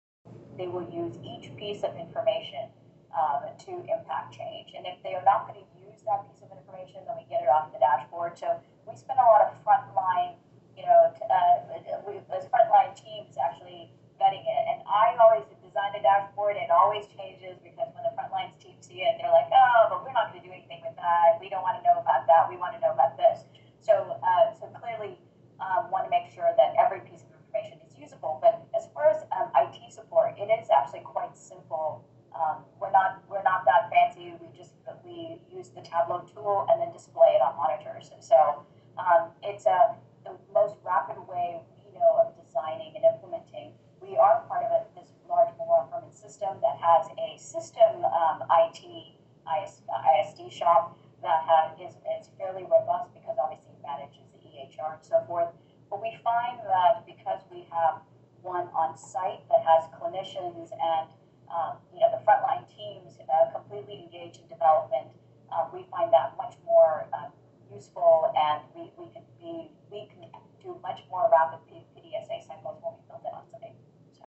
tableau dashboard for healthcare
dashboards are coming to your hospital ward/clinic. data informed team pivoting is impending. how we design it to be meaningful to the teams and the community is the opportunity we face.